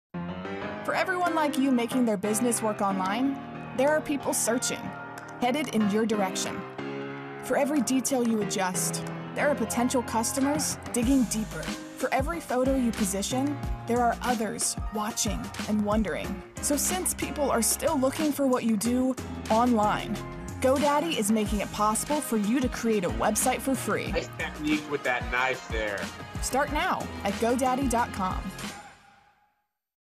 GoDaddy "Food" Commercial (:30, 2020)
This is a GoDaddy commercial that focuses on "Food" sites.